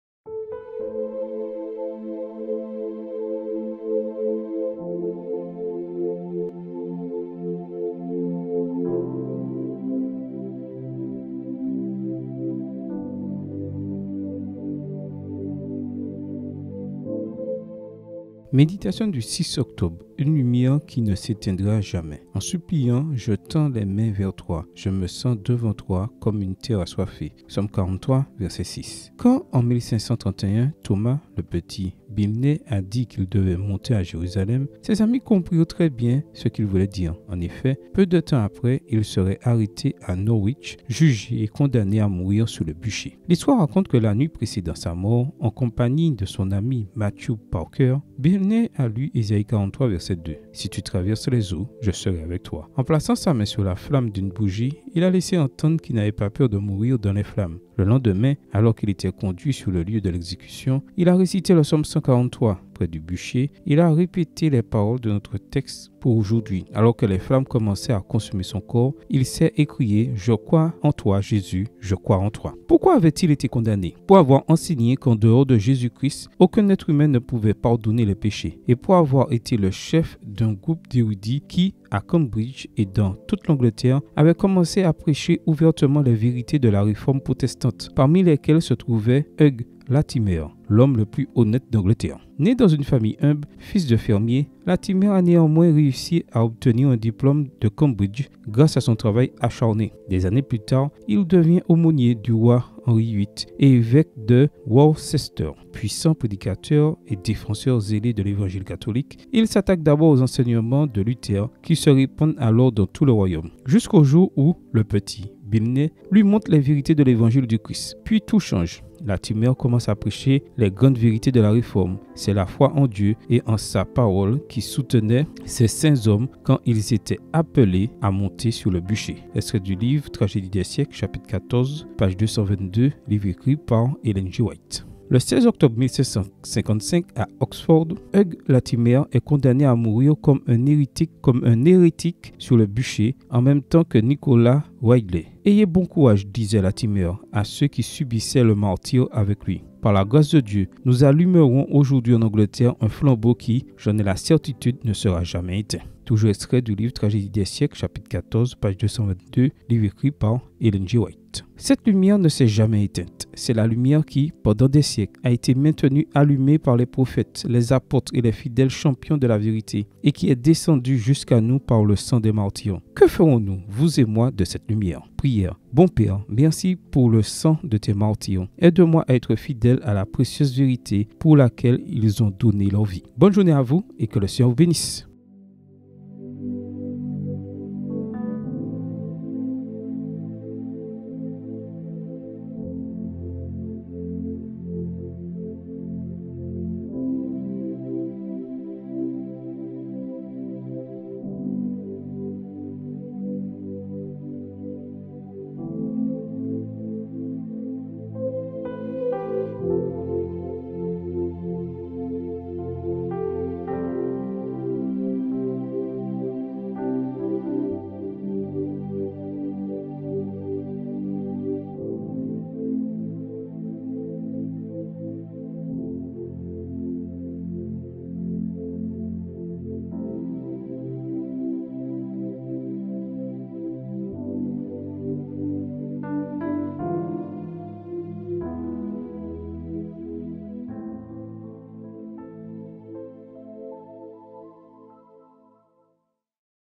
Méditation des Adultes 6 Octobre 2022, Une lumière qui ne s'éteindra jamais
Jeudi 6 Octobre 2022

Prière :

Bon Père, merci pour le sang de tes martyrs.
Aide-moi à être fidèle à la précieuse vérité pour laquelle ils ont donné leur vie !